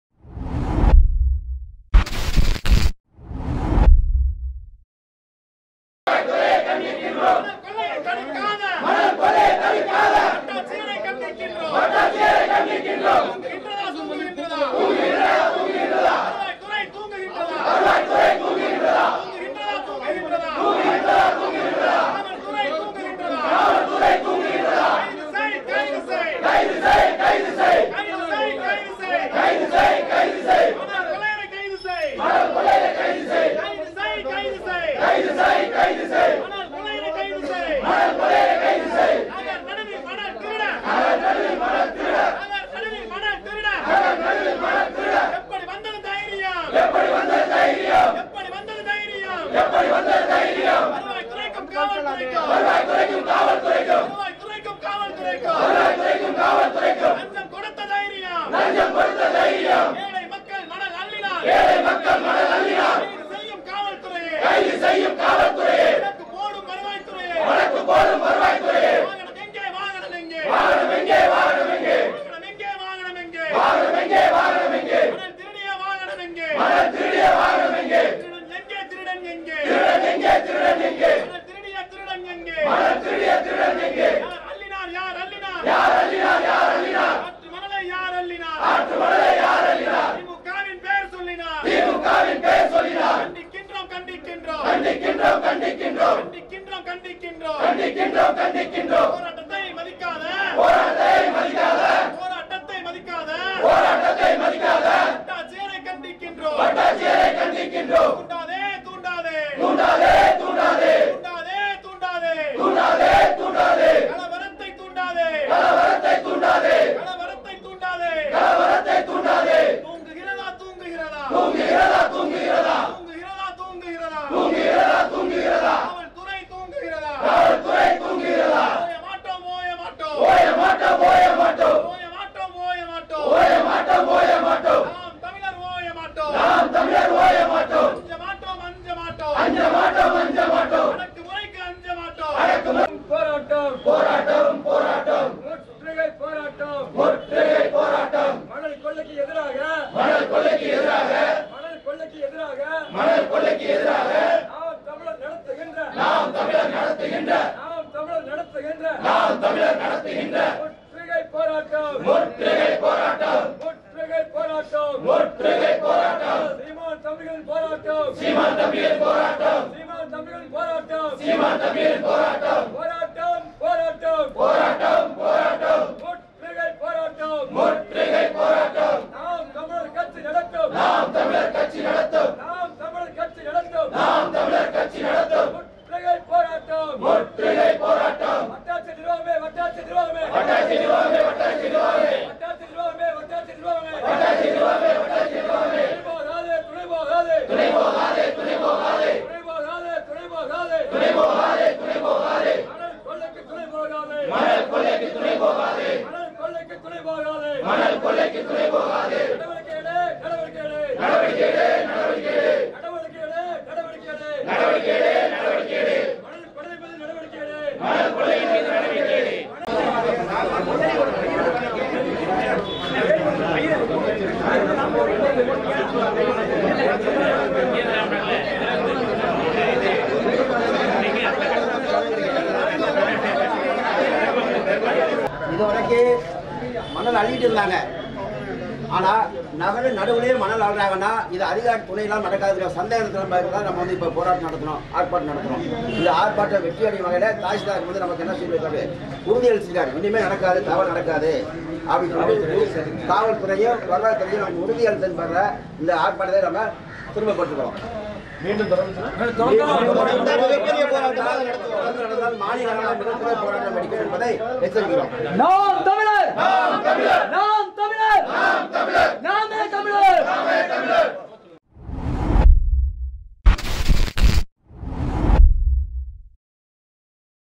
பரமக்குடியில் முக்கியமான இடத்தில் மண் பெருவாரியாக திருடப்பட்டுள்ளது இந்த செயலை கண்டித்துஆர்ப்பாட்டம்
பரமக்குடி நகரின்  மையப்பகுதியான பெருமாள் கோவில் படித்துறைக்கு நேராக உள்ள இடத்தில் மண் பெருவாரியாக திருடப்பட்டுள்ளது மக்கள் நடமாட்டம் அதிகமாக உள்ள முக்கியமான இடத்தில் நடைபெற்ற இந்த கொடுஞ்செயலை கண்டித்து  ஆர்ப்பாட்டம்